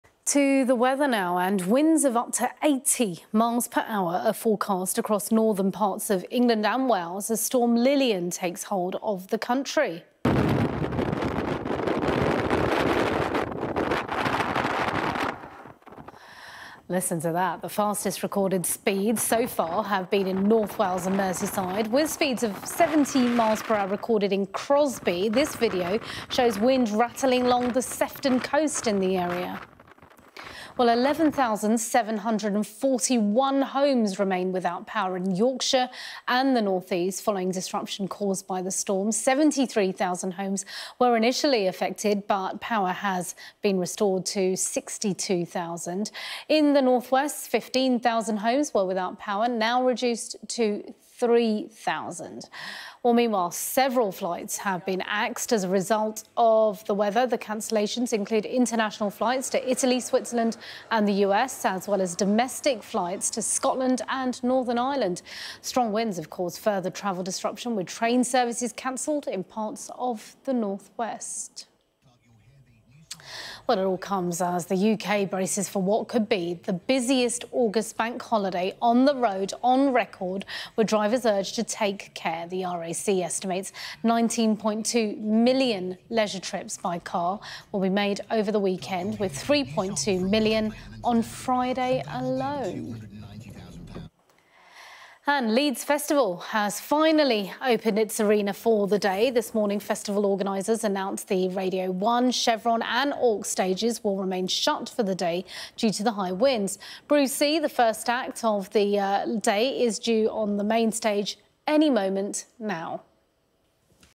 Storm Lilian brings heavy rain and 70mph winds
Winds of up to 80 miles per hour are forecast across northern parts of England and Wales as Storm Lilian takes hold of the country.

The strong winds have caused further travel disruption with train services cancelled in parts of the North West.

It comes as the UK braces for what could be the busiest August bank holiday on the road on record with drivers urged to take care.